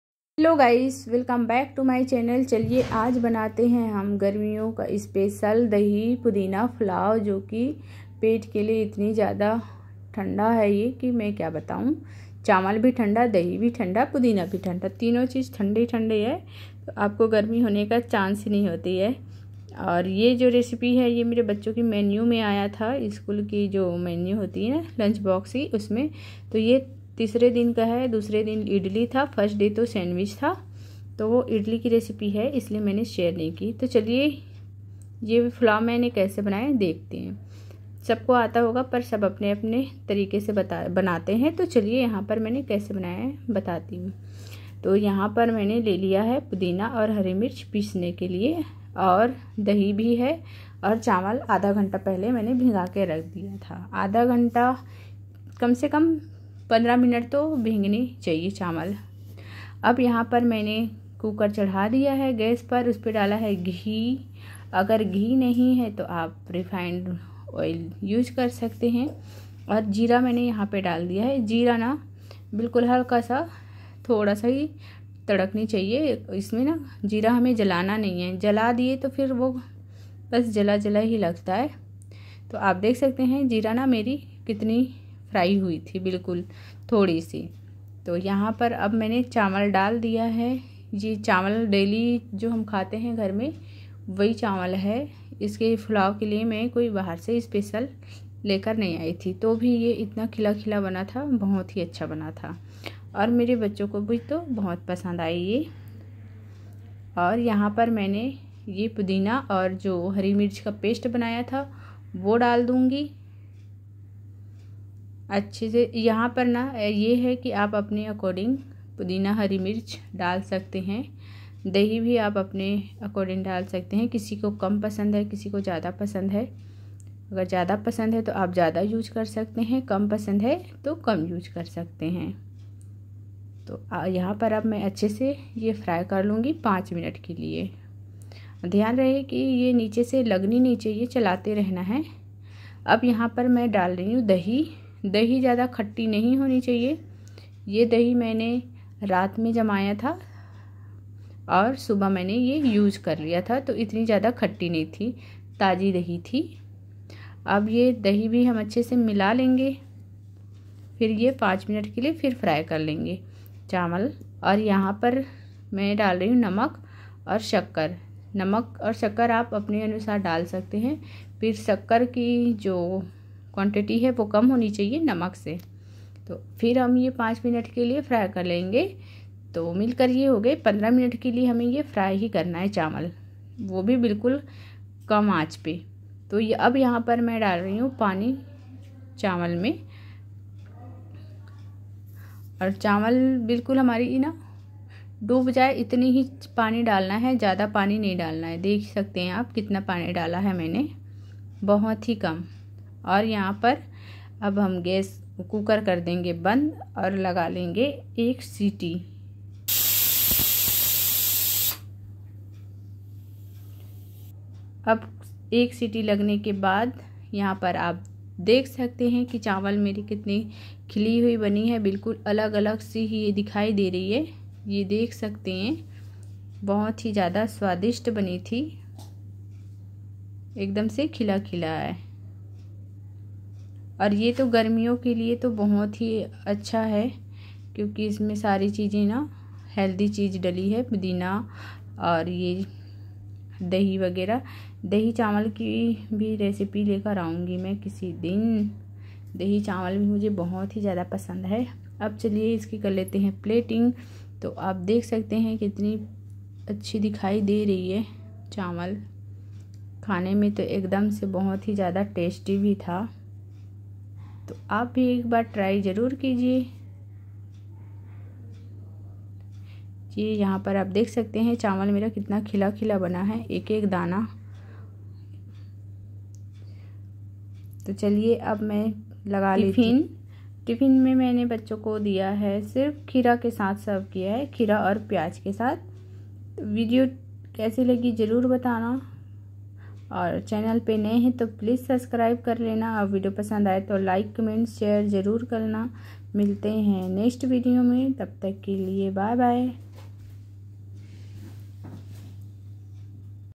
दही पुदीना फुलाव एक बार खाने से पूरी गर्म रहे आपके शरीर ठंडा
दही पुदीना फुलाव एक बार खाने से पूरी गर्म रहे आपके शरीर ठंडाpudin rice recipe